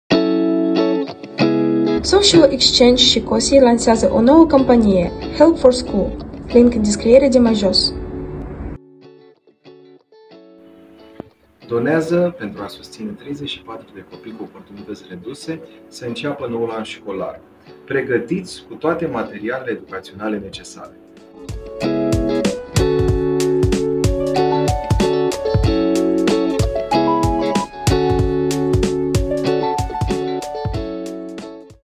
Help4School
We, COSI and SocialXchange, are here to share once again about our campaign - Help4School August.

Our aim is to support the education of 34 disadvantaged Romanian children aged between 5-12. We believe that with your support, we can achieve even more. We are in need of essential school supplies like backpacks, notepads, and pen cases to prepare them for the upcoming school year. Let's come together to work for the future for these children.

💰The EPVP- Eastern Partnership Volunteering Program 4.1®️ project is funded by the European Solidarity Corps program of the 🇪🇺 European Commission!

Noi, COSI și SocialXchange, suntem încântați să anunțăm noua noastră campanie - Help4School August.

Scopul nostru este să sprijinim educația a 34 de copii defavorizați cu vârsta cuprinsă între 5 si 12 ani. Credem că, cu sprijinul vostru, putem realiza și mai mult. Avem nevoie de rechizite școlare esențiale, cum ar fi rucsacuri, caiete, penare și pixuri pentru a le pregăti pentru anul școlar viitor. Donațiile tale pot face cu adevărat diferența. Să ne unim pentru a lucra pentru viitorul acestor copii. Campania online este aici

💰 Proiectul EPVP - Eastern Partnership Volunteering Program 4.1 ®️ este finanțat de programul Corpul European de Solidaritate al 🇪🇺 Comisiei Europene!